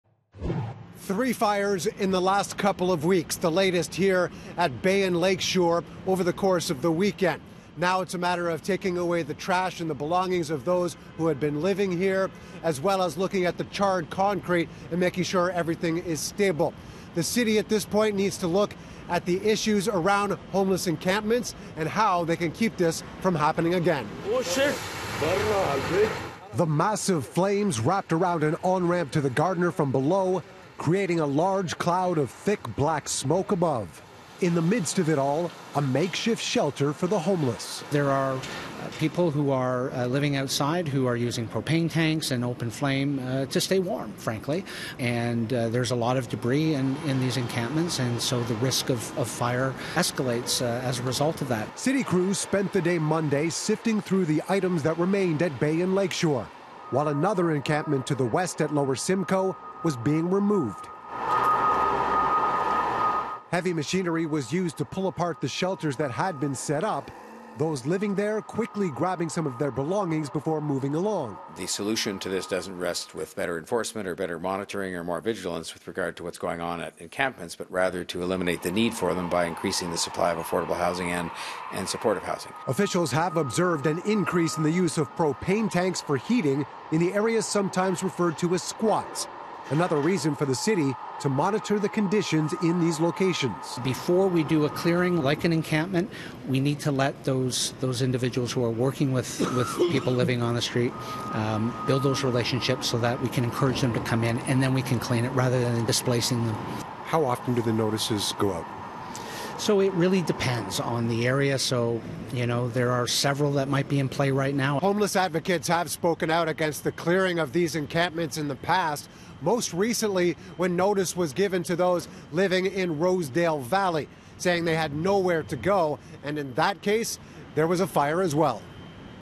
City dealing with third fire at homeless encampment in recent weeks
There have been three fires at homeless encampments over the past several weeks in Toronto. Mark McAllister with what the city is doing to keep those on our streets safe, and how they are helping those displaced.